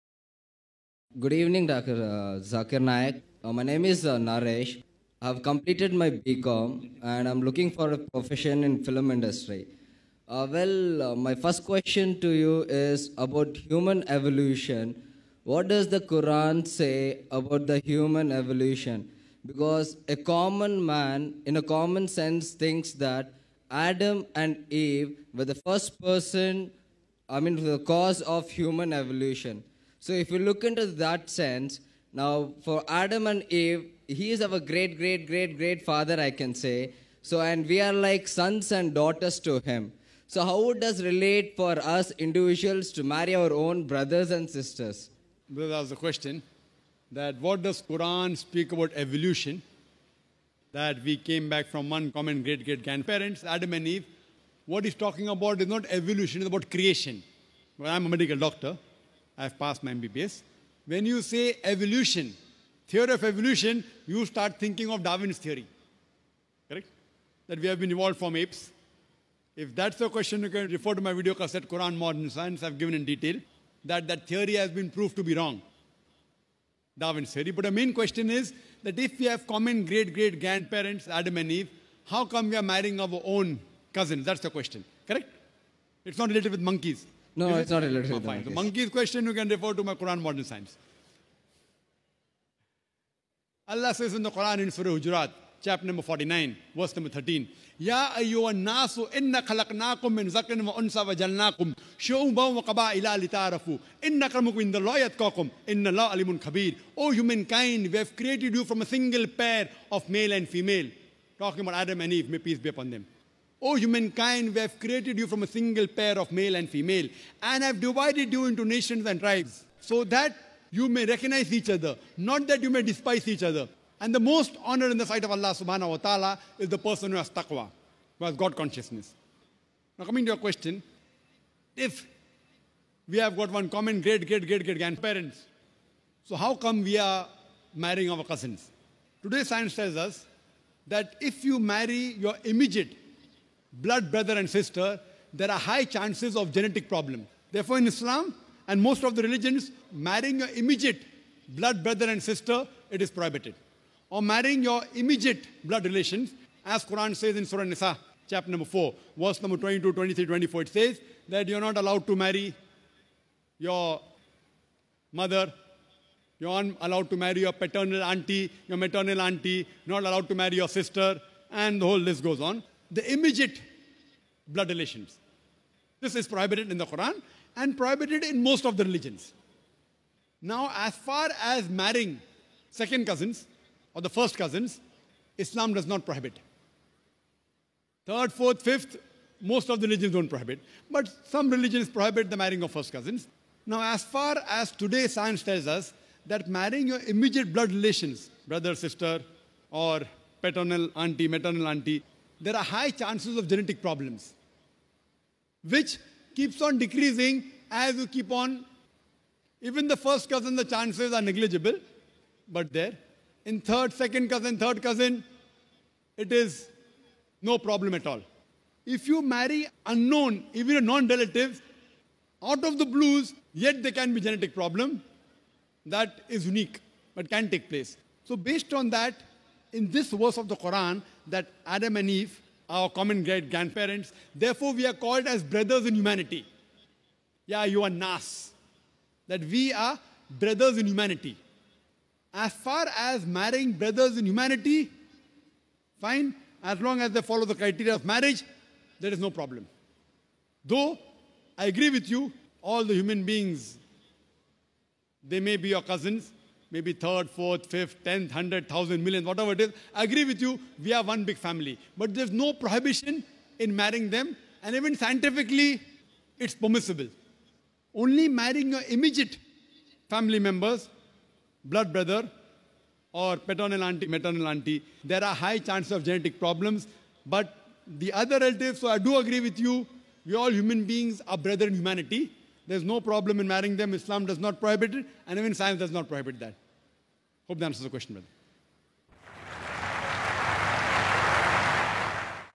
If Adam (pbuh) and Eve (pbuh) are our Great Great Grandparents... - Dr Zakir Naik
If Adam (pbuh) and Eve (pbuh) are our Great Great Grandparents, we all Human Beings are Brothers and Sisters then how can we Marry each other? - Dr Zakir Naik